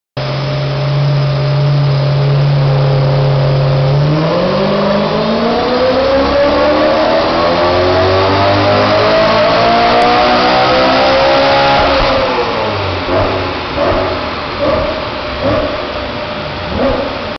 BMW E92 M3 AA X Pipe Tubi Exhaust
BMW E92 M3 with AA X Pipe and Tubi Rear Exhaust  and remap on evolve Dyno Dynamics